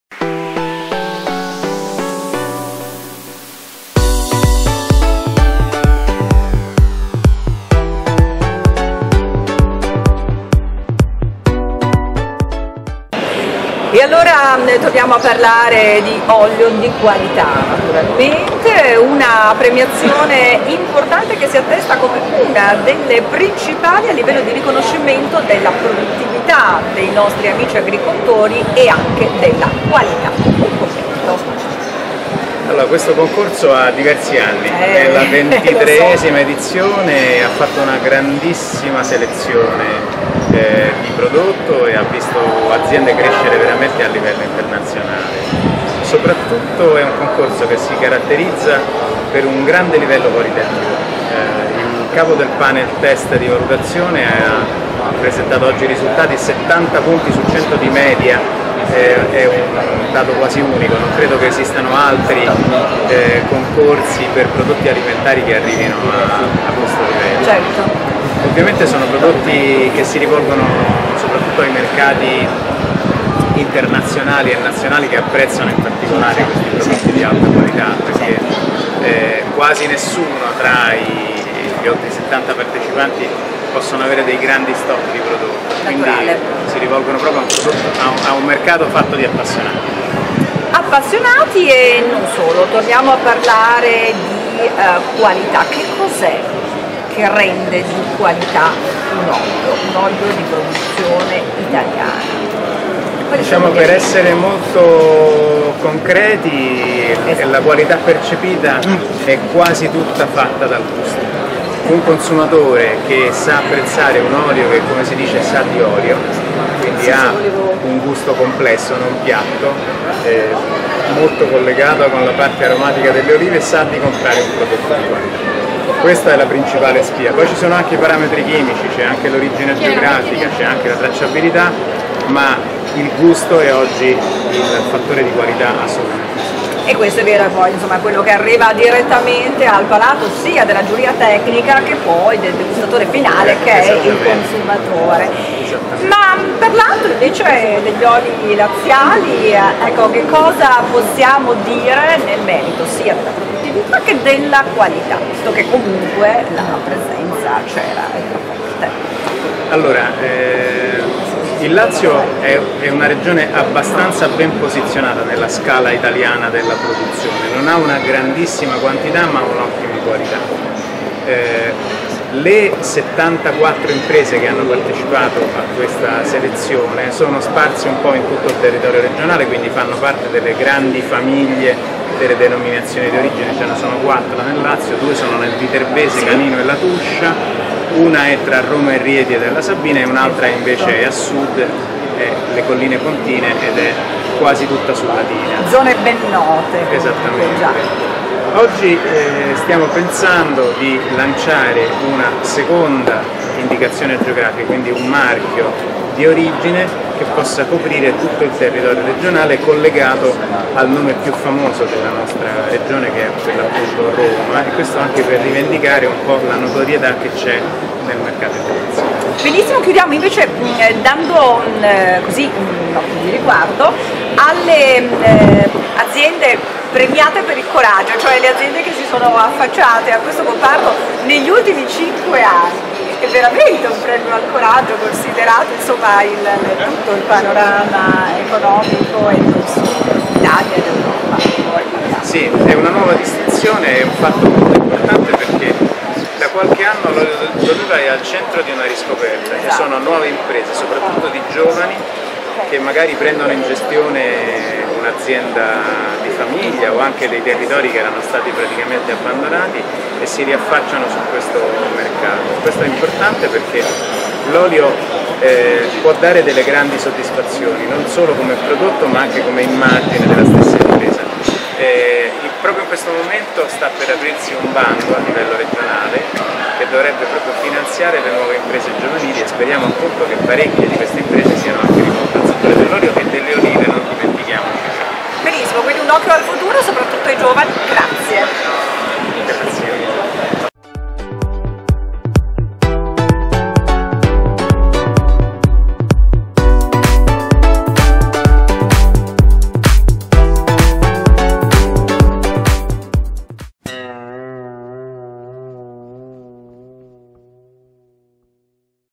Orii del Lazio - Carlo Hausmann, Assessore all'Agricoltura Regione Lazio
XXIII Concorso regionale per i migliori oli extravergine di oliva.
Tempio di Adriano, in Piazza di Pietra a Roma.

Intervista di CAMILLA NATA a CARLO HAUSMANN,  Assessore all'Agricoltura Regione Lazio.